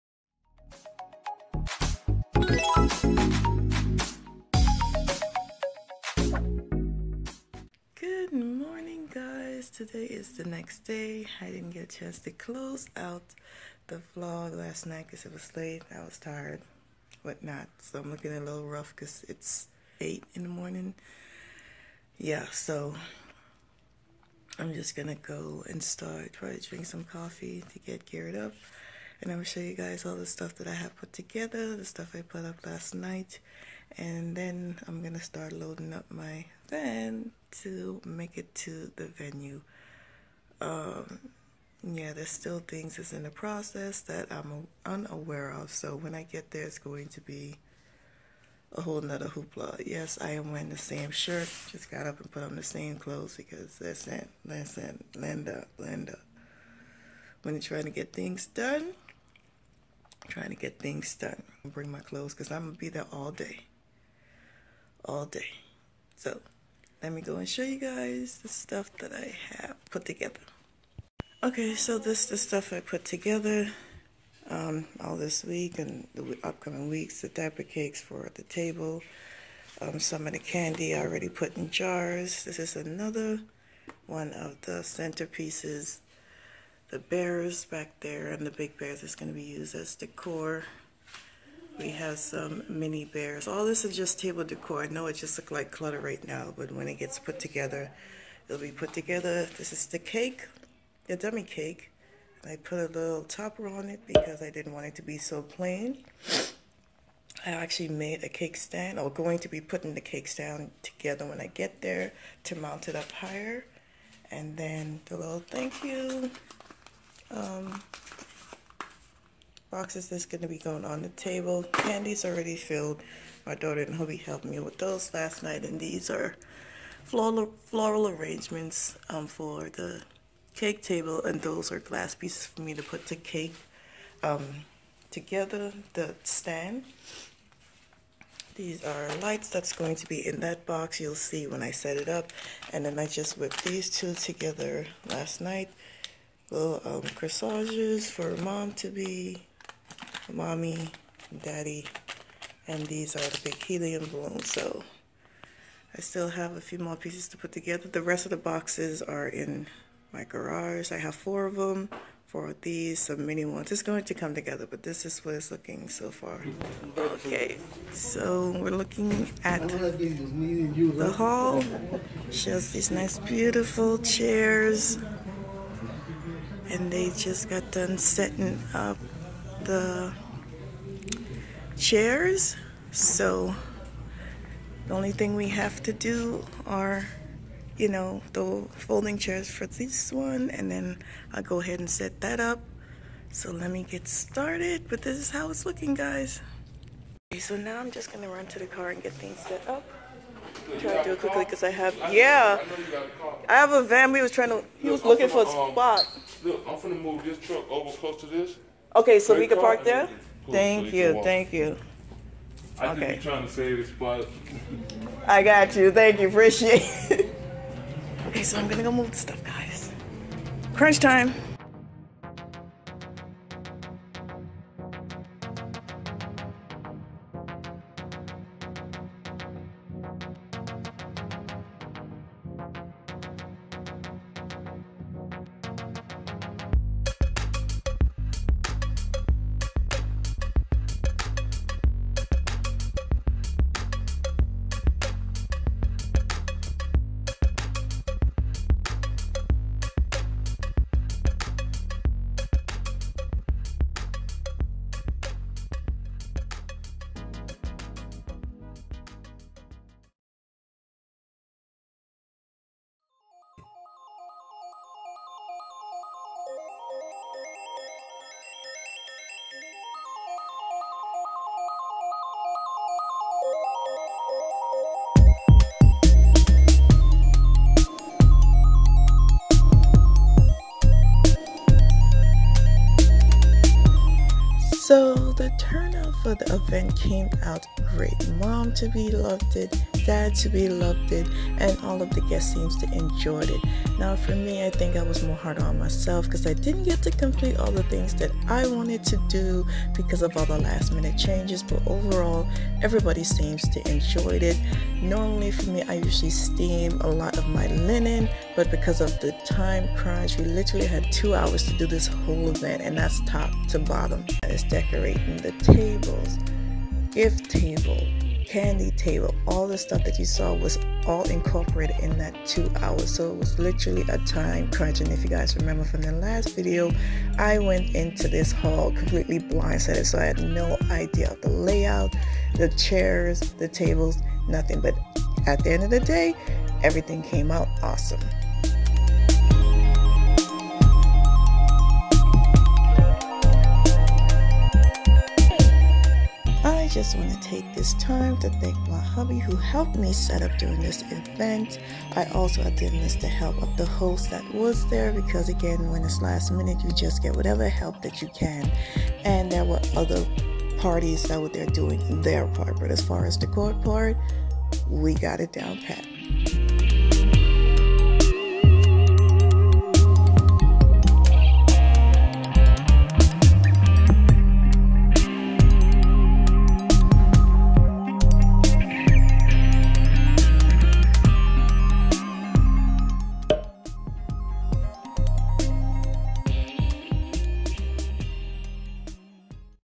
Little Man Baby Shower Setup and Reveal
Hey Guys!

The long await is over. it's time for the reveal! So the theme of the shower is Little Man incorporating teddy bears. We had 2 hours to complete the decor in this event, so it was a real time crunch to get it all done before guest arrive.